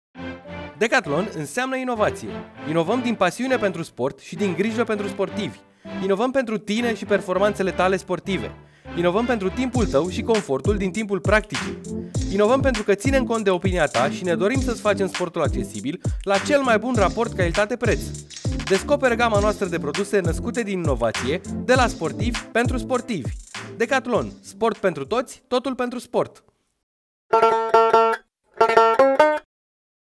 Inovație, de la sportivi, pentru sportivi!
Inovăm permanent pornind de la feedback-ul primit de la voi, sportivii noștri, și ne dorim să vă propunem produse unice pe piață, mai tehnice, cu ajutorul cărora să practicați sport mai ușor și mai confortabil.